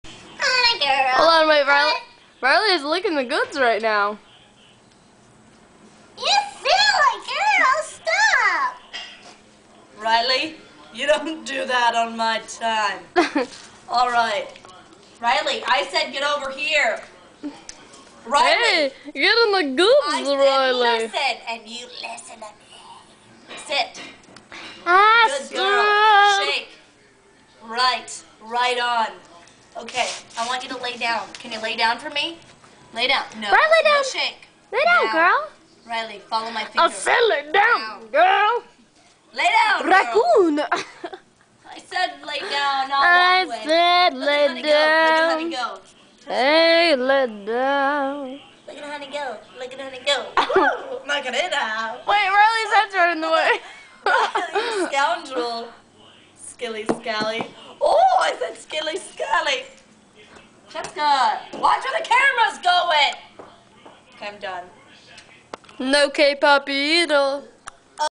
danielle and friends
playing with the doggies